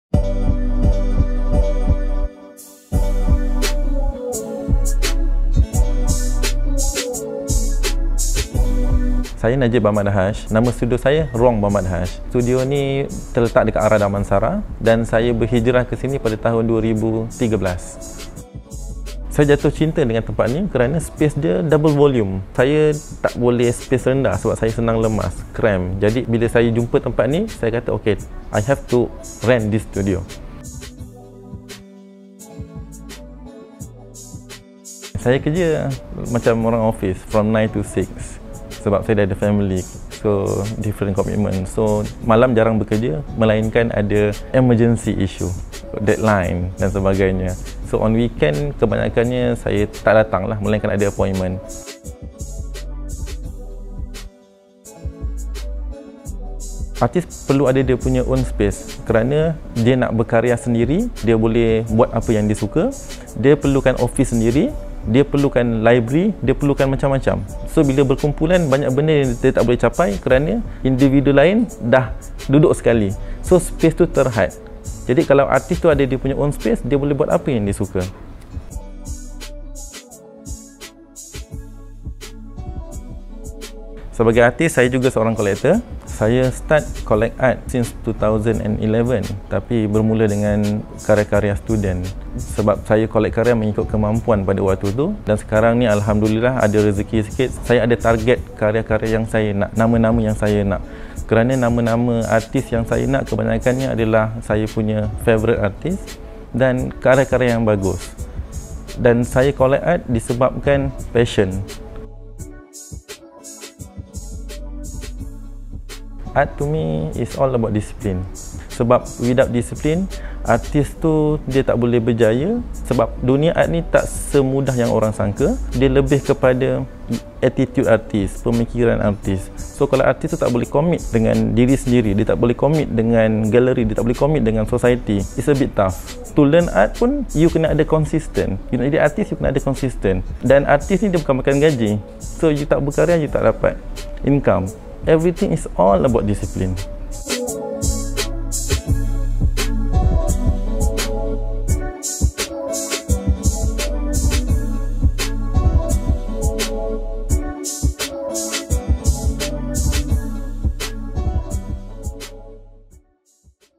In the Studio with Najib Bamadhaj | The Artist's Studio | G13 Gallery
G13 Gallery is pleased to present “In the Studio with Najib Bamadhaj”, a part of our interview video series in conjunction with the release of “The Artist's Studio: Exploring the Workspace of 17 Malaysian Artists,” a 300-page publication that offers a personal journey into the private workspaces of 17 Malaysian artists. In this video, Najib talks about the importance of studio for artists, discussing his art collection journey, and highlighting the crucial role of discipline in the art world. For a more in-depth understanding, be sure to watch the video!

Join us on 2 December 2023 (Saturday) at G13 Gallery to celebrate the book launching and the group exhibition. The group exhibition runs from 2 to 23 December 2023.